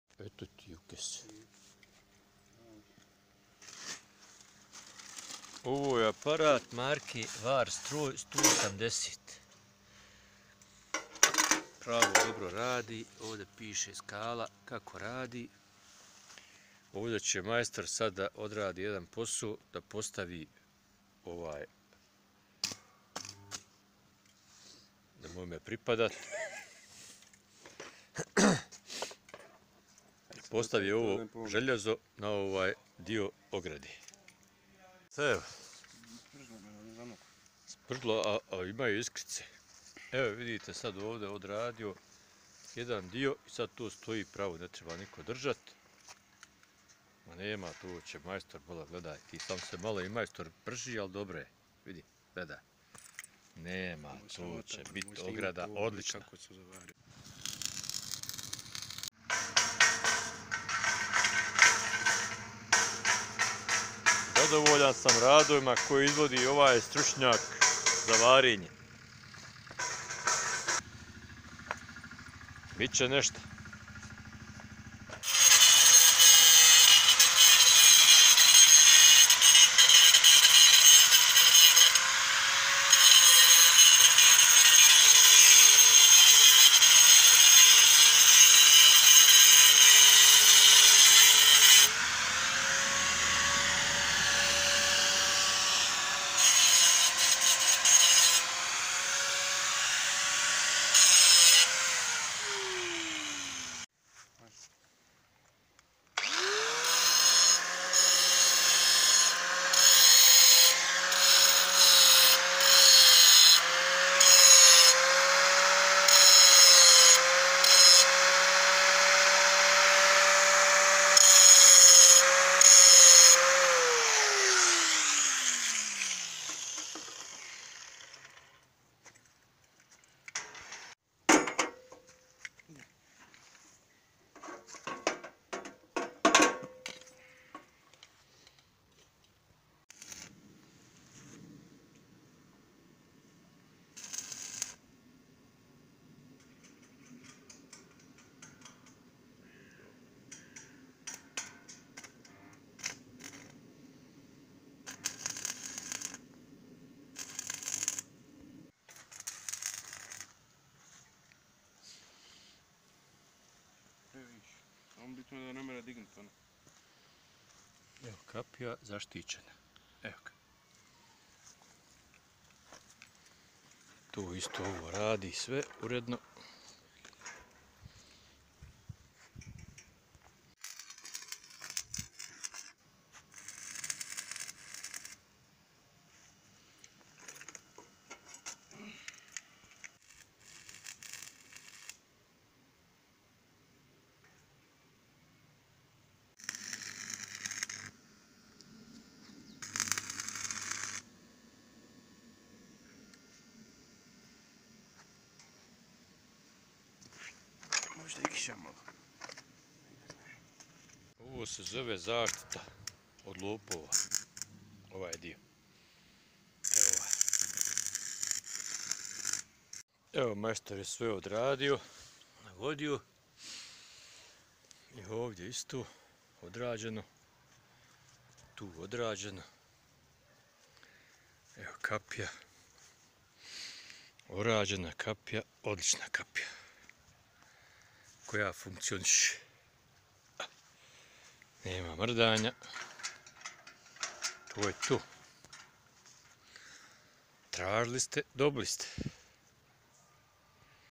Varenje kapije aparatom marke Varstroj varex 180
Korišten je aparat za varenje marke VARSTROJ VAREX 180.Mladi varioc je napravio kapiju kao profesionalac.